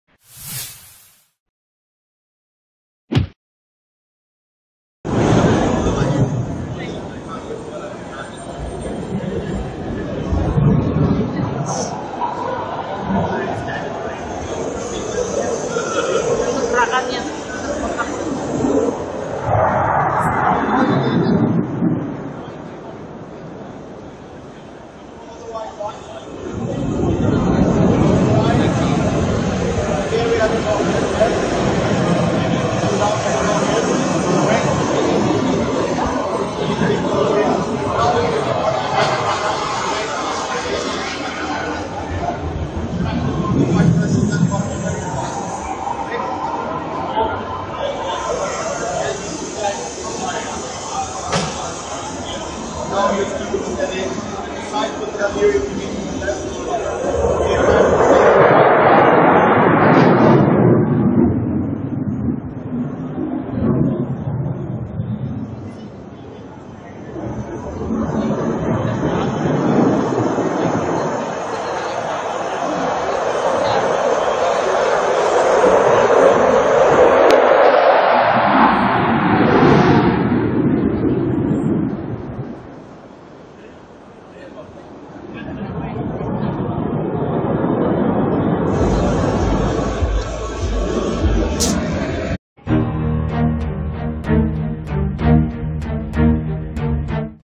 Cockpit View - Atraksi Aerobatik Jet Tempur Gripen di Aero India 2017
Video ini memperlihatkan cokpit view dari atraksi aerobatik jet tempur Saab JAS39 C Gripen di pameran dirgantara Aero India 2017 yang berlangsung di Bangalore 13 - 18 Februari 2017.